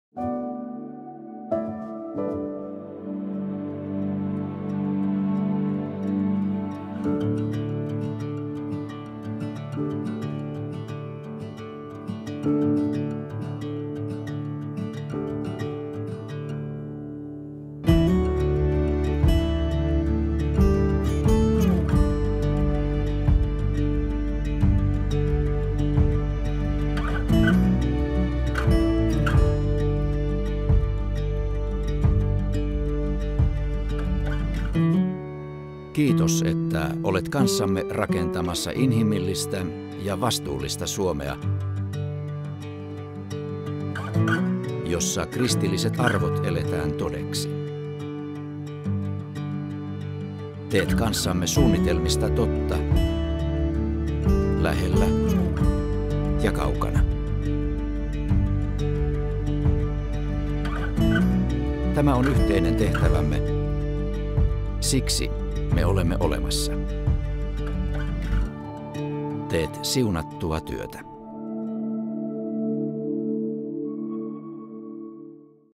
Teet siunattua työtä - kiitos!
Kiitos, että olet kanssamme rakentamassa inhimillistä ja vastuullista Suomea, jossa kristilliset arvot eletään todeksi. Teet kanssamme suunnitelmista totta lähellä ja kaukana. Tämä on yhteinen tehtävämme, siksi me olemme olemassa.
Kirkkohallitus kiittää tekemästäsi siunatusta työstä.